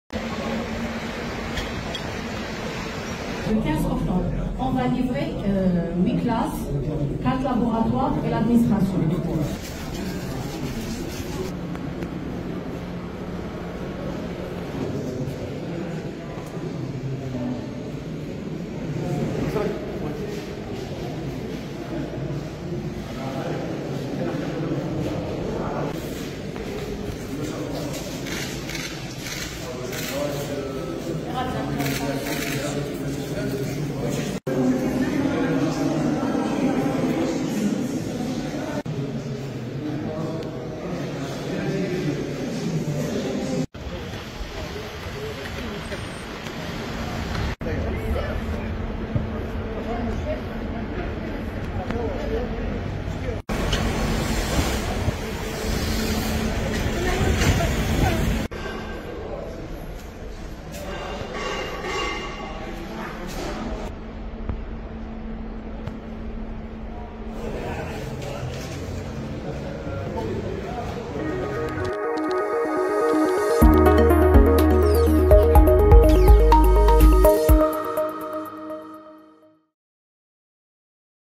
🔴شاهد...وزير التربية عبد الحكيم بلعابد ووالي وهران يعاينان أشغال إعادة تهيئة ثانوية جهوية للرياضيات
🔴شاهد...وزير التربية عبد الحكيم بلعابد و والي ولاية وهران سعيد سعيود يعاينان أشغال إعادة تهيئة المقر الذي سيحتضن ثانوية جهوية للرياضيات و التي ستستقبل تلاميذا من 19 ولاية من غرب و جنوب غرب البلاد